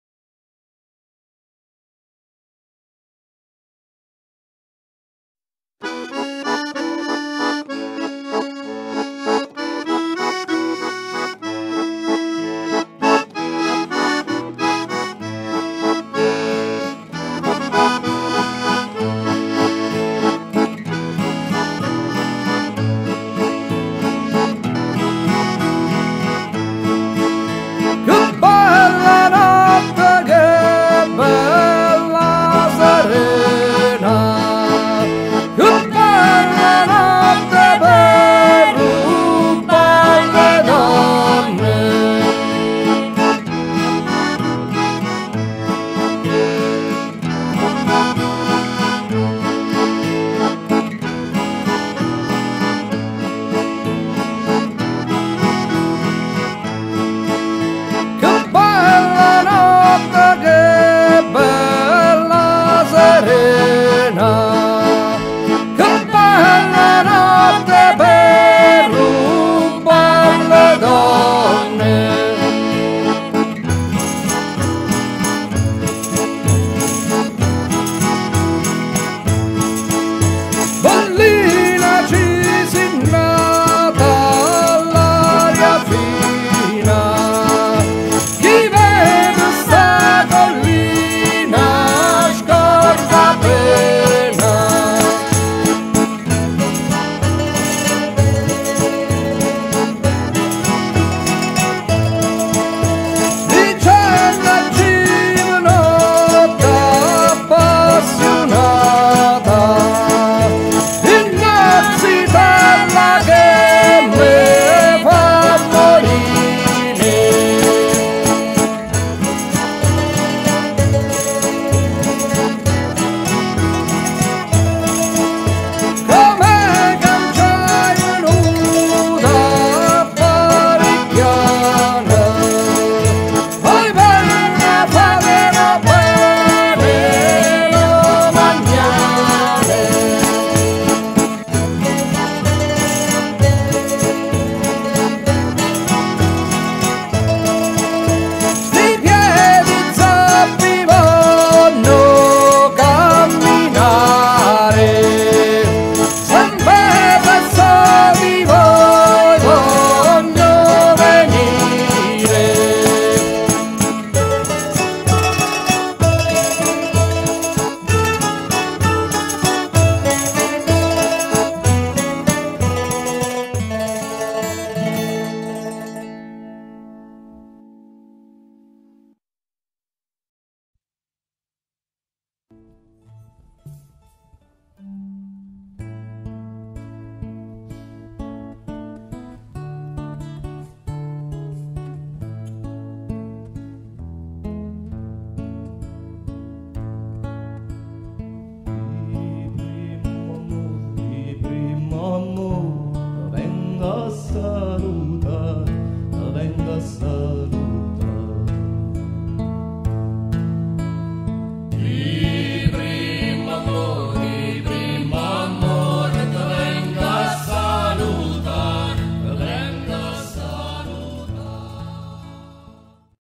Briganti di Frontiera x Stefano
Dedicato al chitarrista  Stefano Moschetti prematuramente scomparso in questo 2019 : una breve ricostruzione della storia della band con due brani tradizionali eseguiti dai Briganti